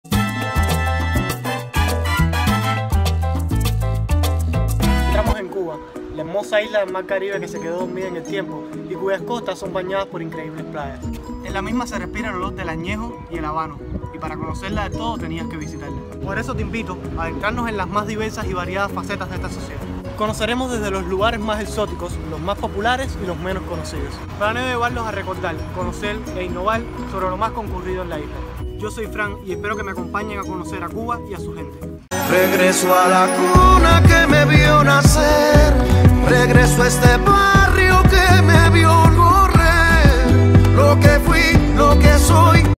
RECORRIENDO TODA CUBA
Cuba, la hermosa isla del Caribe dormida en el tiempo, la que es bañada por  increíbles playas, y en la que se respira el olor de añejo y habanos. Hasta ahora para conocerla del todo, tenias que visitarla. Por eso te invito a  adentrarnos en las mas diversas y variadas fascetas d la sociedad,cultura y diversión de cuba. Desde los lugares mas exóticos, los mas populares y los menos conocidos.  Planeo llevarlos a recordar,conocer e innovar acerca d lo mas concurrido en la isla, desde  sus paisajes hasta su historia.  Soy frank, y espero me acompañes a conocer a Cuba y su gente.

Nos vemos en unos dias con una aventura nueva